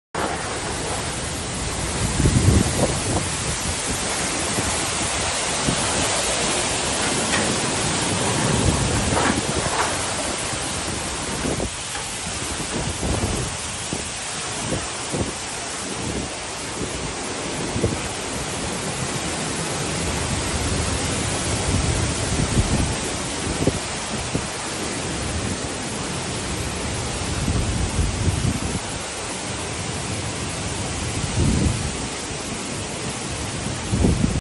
Fuertes vientos en Luisiana por el Huracán Ida EEUU Agosto 2021
Los fuertes vientos que se registraron en Lousiana por el huracán Ida, hicieron volar el techo de una casa